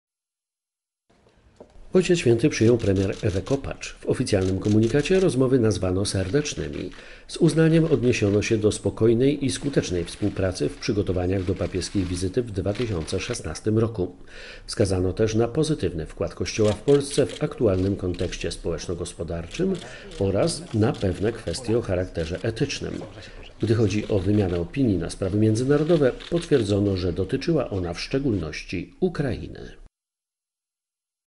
Premier Ewa Kopacz w Watykanie
Ojciec Święty przyjął premier Ewę Kopacz. W oficjalnym komunikacie rozmowy nazwano „serdecznymi”. Z uznaniem odniesiono się do „spokojnej i skutecznej współpracy” w przygotowaniach do papieskiej wizyty w 2016 r. Wskazano też na pozytywny wkład Kościoła w Polsce „w aktualnym kontekście społeczno-gospodarczym” oraz „na pewne kwestie o charakterze etycznym”. Gdy chodzi o wymianę opinii na sprawy międzynarodowe, potwierdzono, że dotyczyła ona w szczególności Ukrainy.